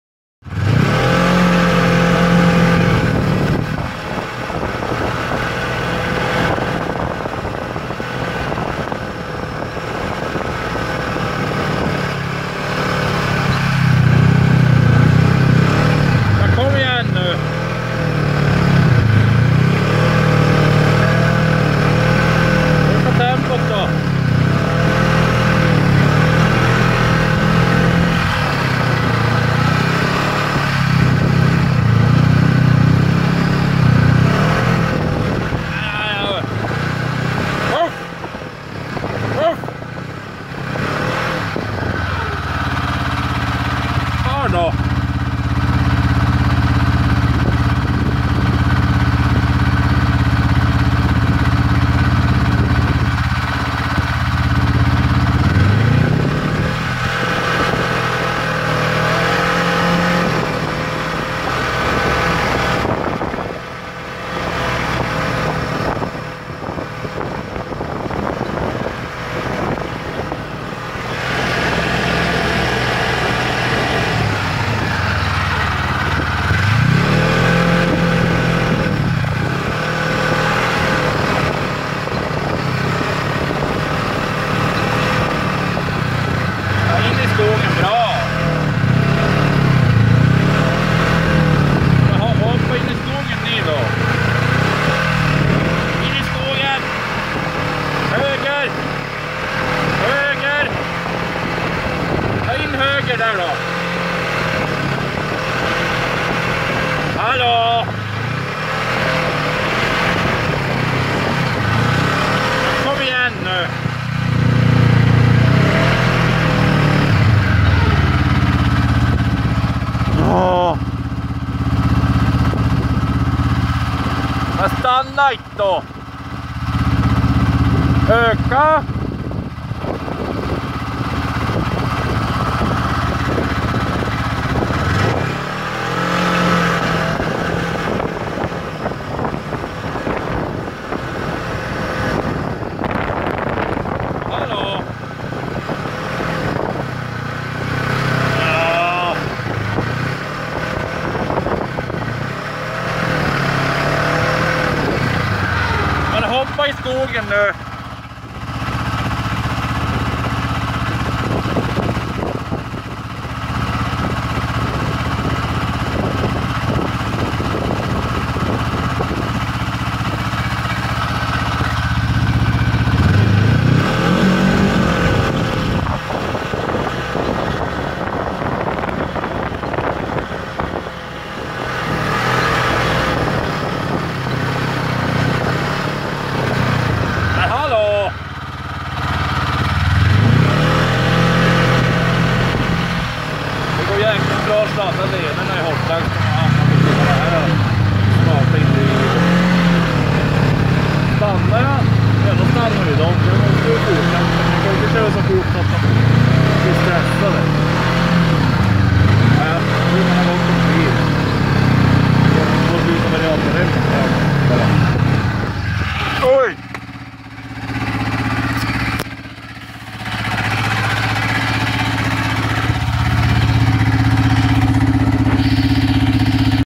Renar på skoterleden
Jag var ute och sladdade lederna kring Hortlax när ett gäng fyrfota vänner dök upp och ville trampa upp ett spår som jag kunde följa ;)
Det slutar med att jag kör ner i ett hål, men det gick bra :) då jag kommit upp ur hålet var renarna borta